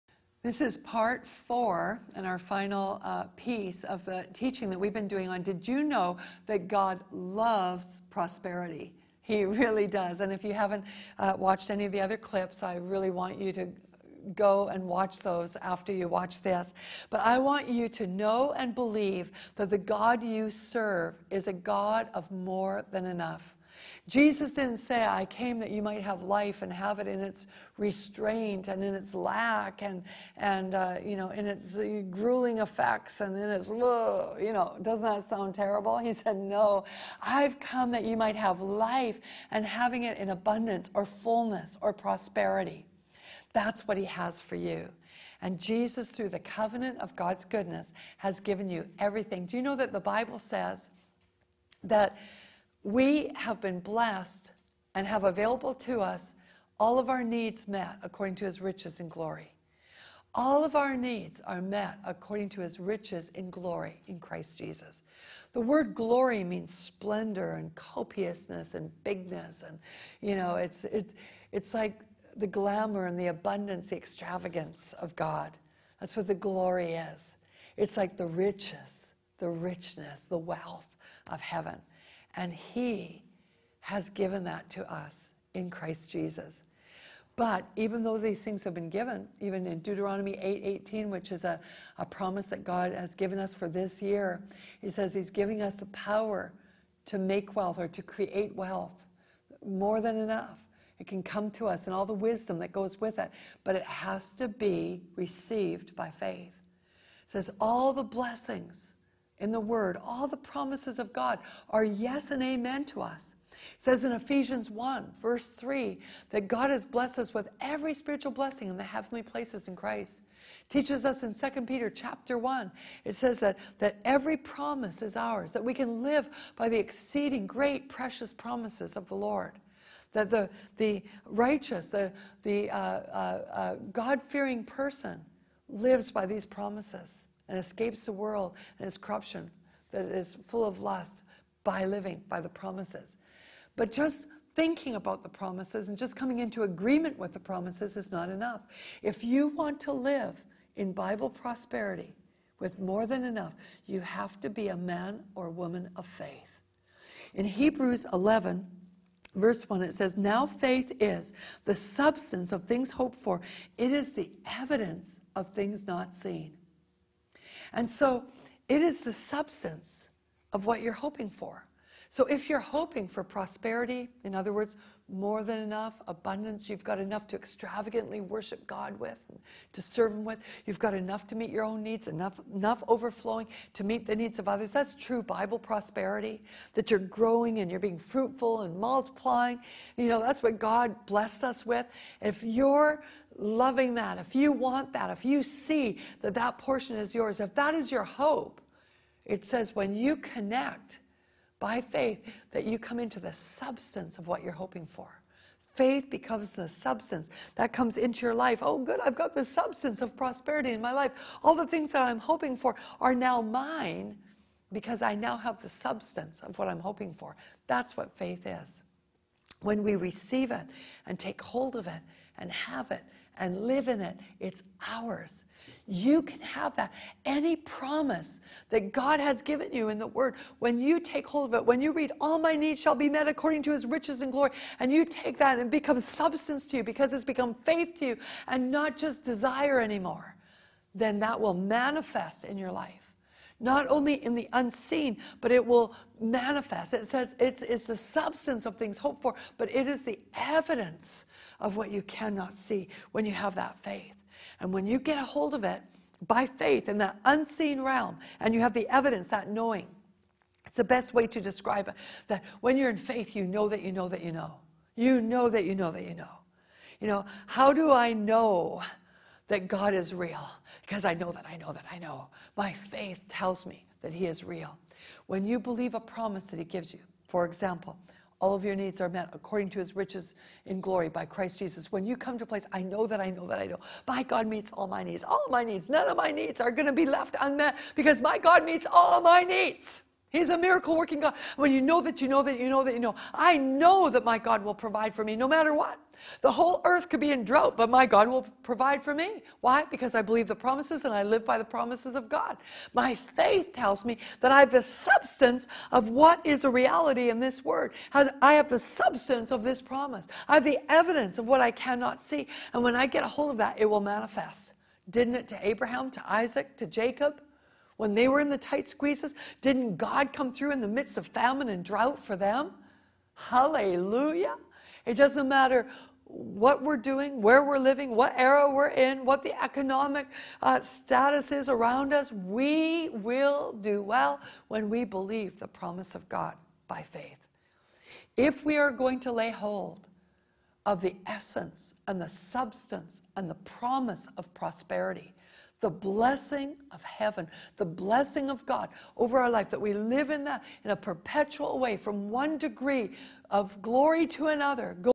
Patricia King: God Loves Prosperity (pt.4.a)
Jesus didnt say He came so we might have life in lack. No. He said He came so we might have life in fullness! The God we serve is a God of more than enough. It is time that we as Christians, begin to not only believe that, but live in it.

In this final installment of her four-part series, God Loves Prosperity, Patricia shares from scripture about the richness, abundance and extravagance of God. He wants to pour it all out on you by giving you the power to make wealth. As Patricia unpacks several key scriptures, you will not only come to understand the promises of God in this area, but will also learn how to receive and activate those promises in your circumstances!